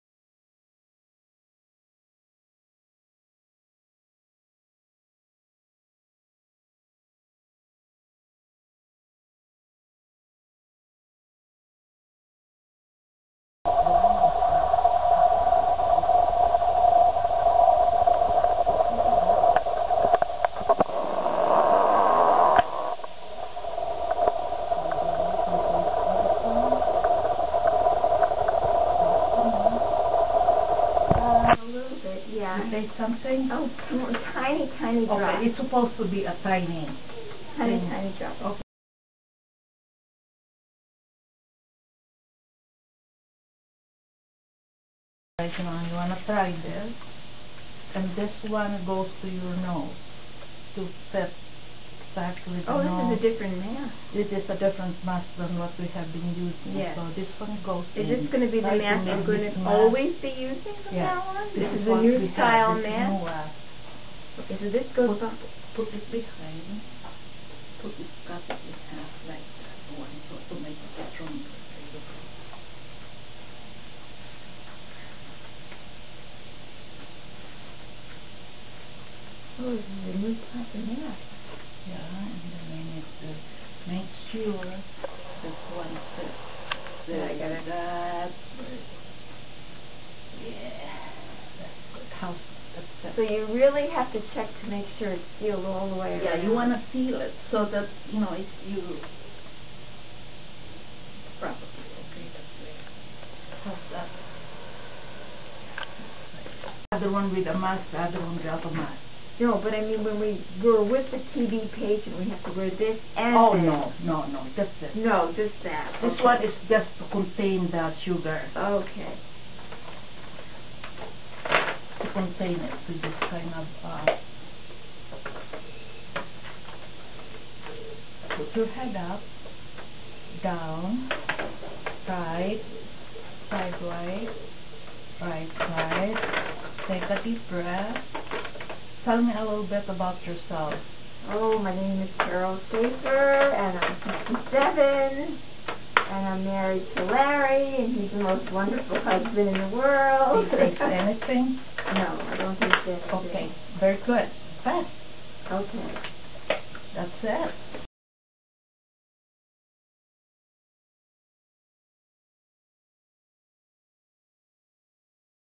Correct Respirator Mask Precautions in Health Care - Air borne contaminants, particulates, TB, etc.
A new respirator that will further aid in the protection from such airborne contagious particulates such as TB.  The sugar spray test with hood on is a very effective and quickly given test to make sure you understand the procedure.